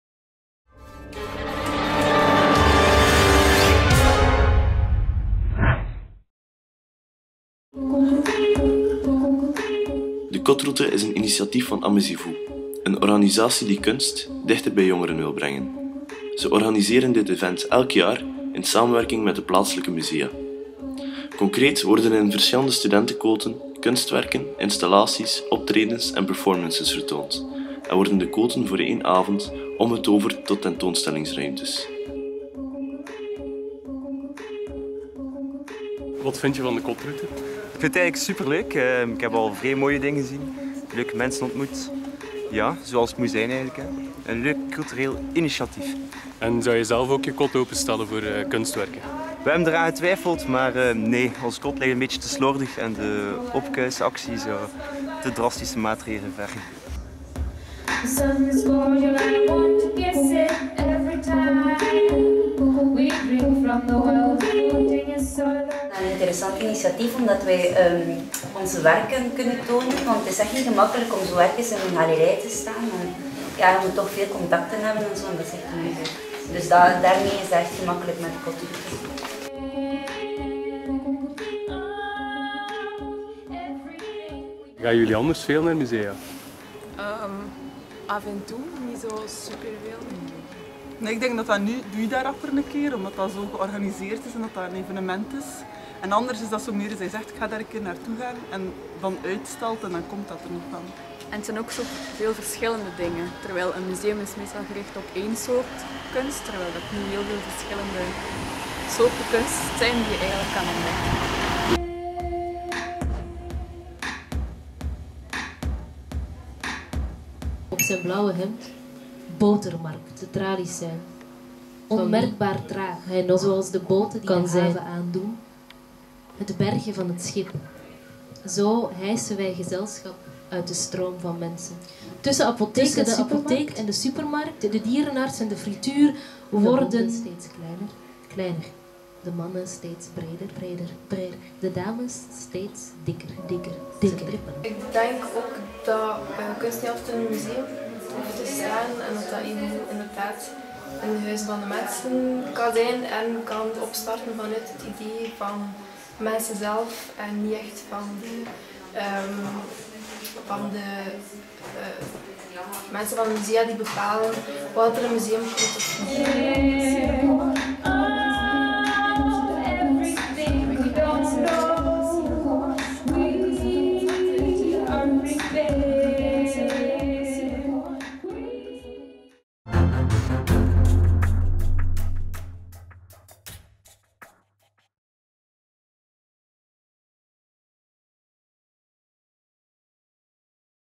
De Kotroute, Een kunstzinnige Avondwandeling in Gent
De kotroute is een initiatief van amusez-vous, een organisatie die kunst dichter bij jongeren wil brengen.

Ze organiseren dit event elk jaar in samenwerking met de plaatselijke musea.
Concreet wordt er in verschillende studentenkoten kunstwerken, installaties, optredens en performances vertoond en worden de koten voor 1 avond omgetoverd tot tentoonstellingsruimtes.

Je kan zelf je route samenstellen om van kot tot kot te reizen en hebt de keuze tussen de steden Gent, Antwerpen, Brussel, Leuven en Hasselt.
wij liepen mee met enkele bezoekers in Gent.

Om de bezoekers, de kotbewoners en de kunstenaars te bevragen naar hun bevindingen.
en natuurlijk om de sfeer wat op te snuiven.